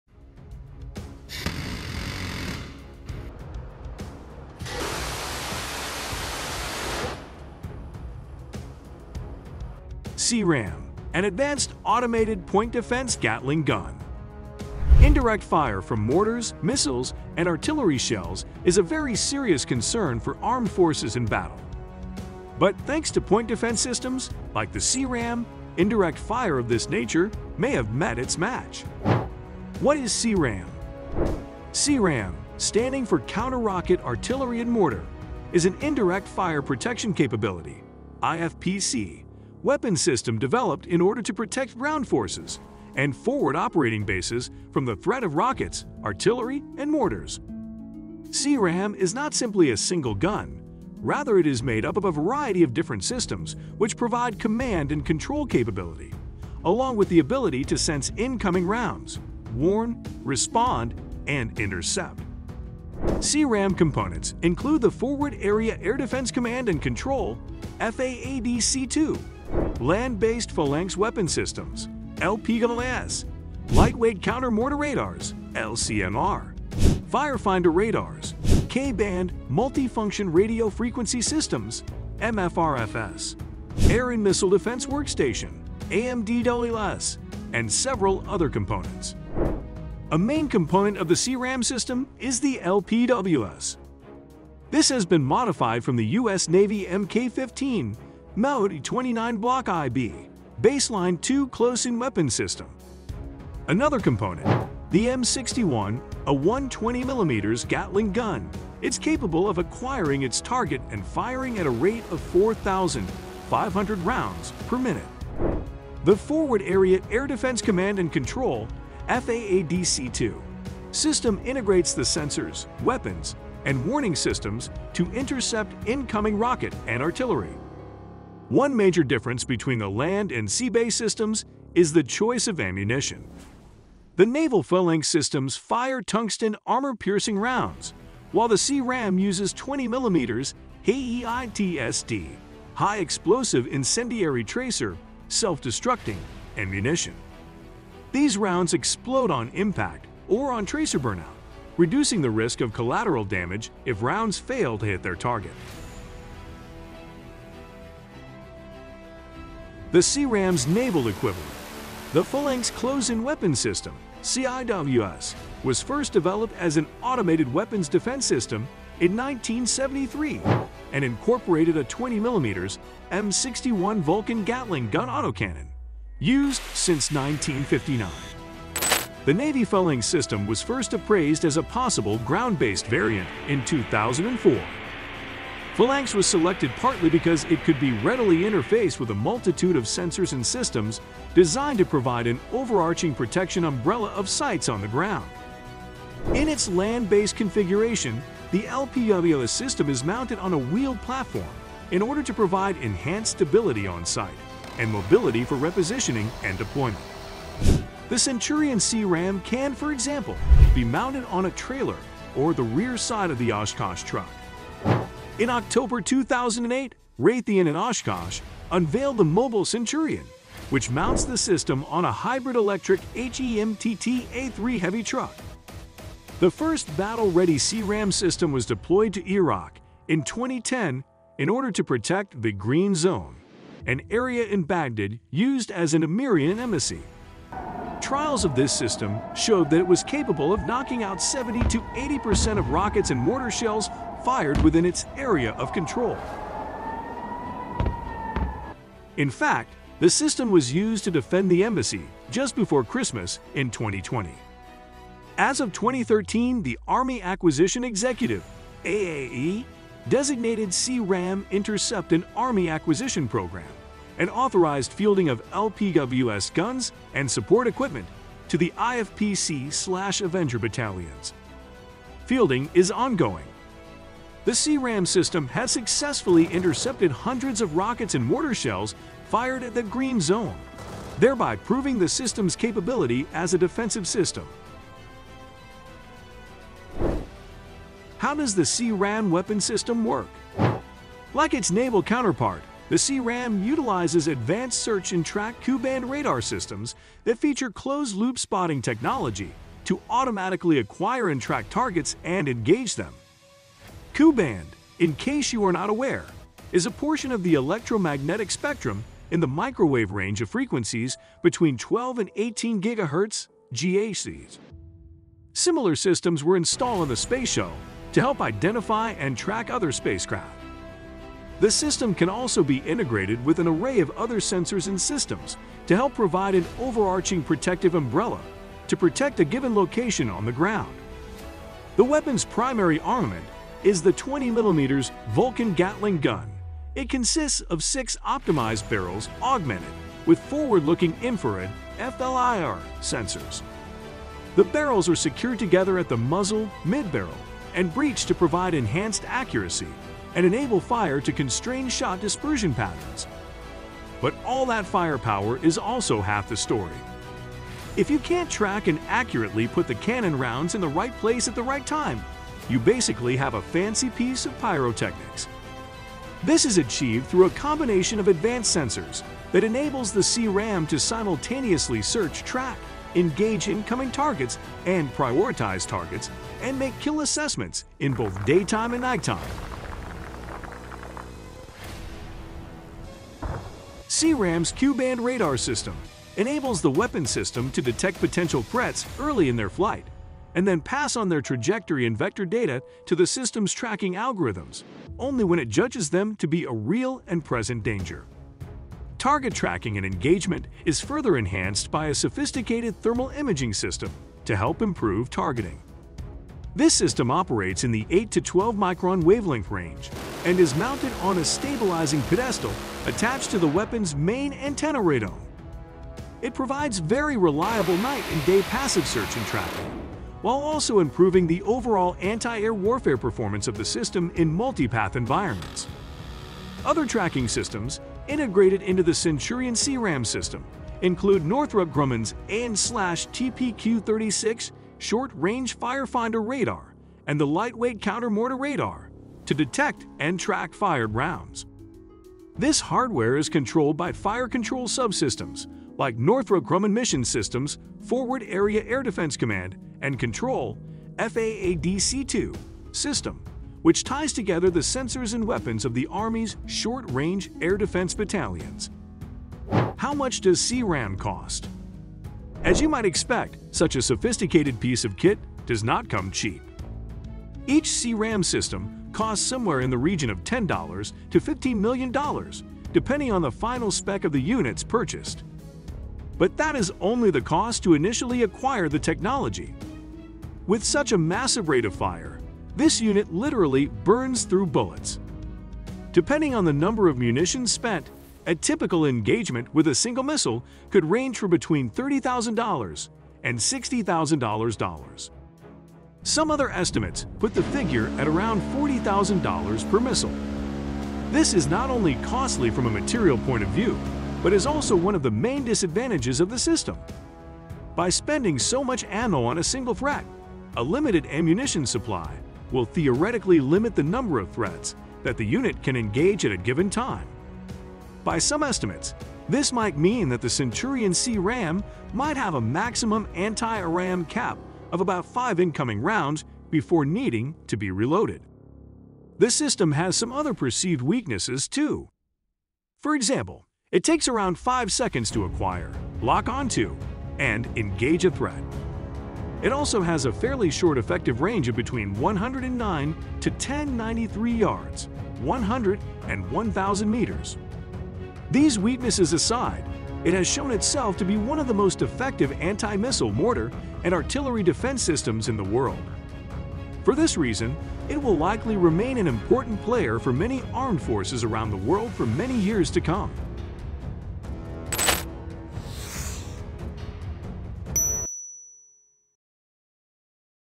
Here's America's New C-RAM Weapon System Shocked The World.
Indirect fire from mortars, missiles, and artillery shells is a very serious concern for armed forces in battle. But, thanks to point-defense systems, like the C-RAM, indirect fire of this nature may have met its match.

What is C-RAM?.